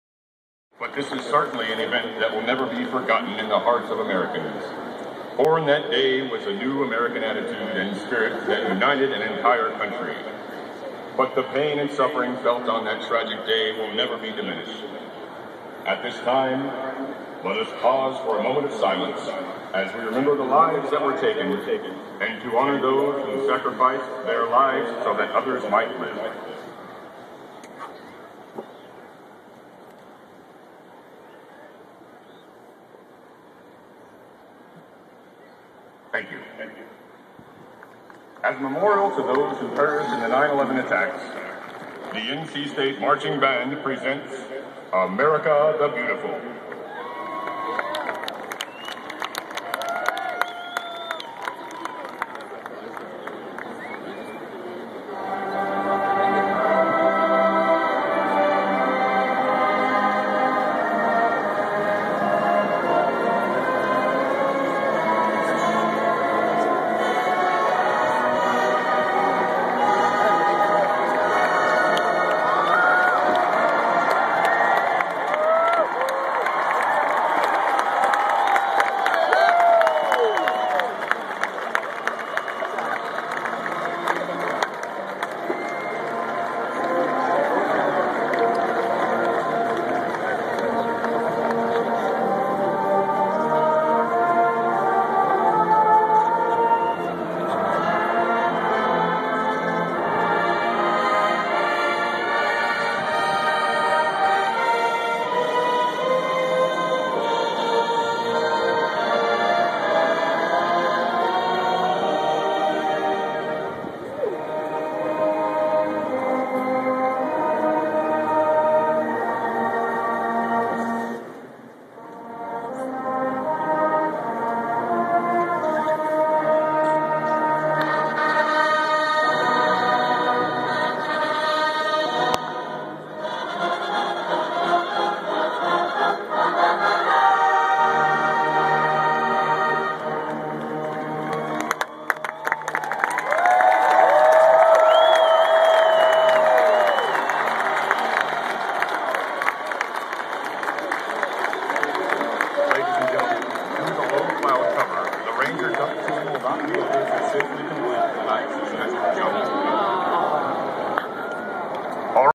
NC State Football Tribute to 9/11/2001
Tribute to 9/11/2001 during halftime of the NC State vs. South Alabama Military Appreciation Day Game on Saturday, September 17, 2011